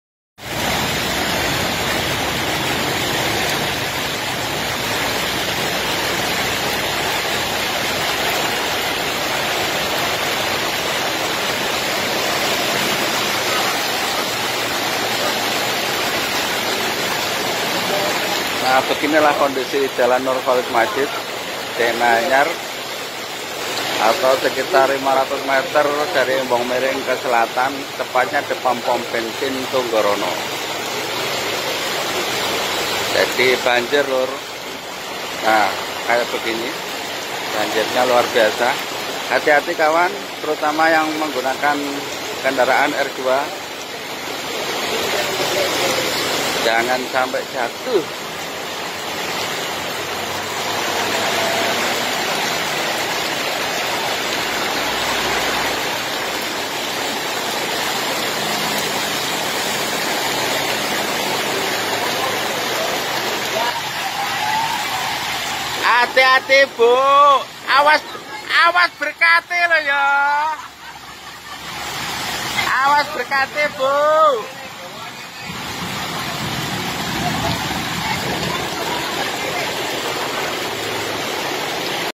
banjir di denanyar,jombang 2/2/2023
telah terjadi banjir daerah denanyar,jombang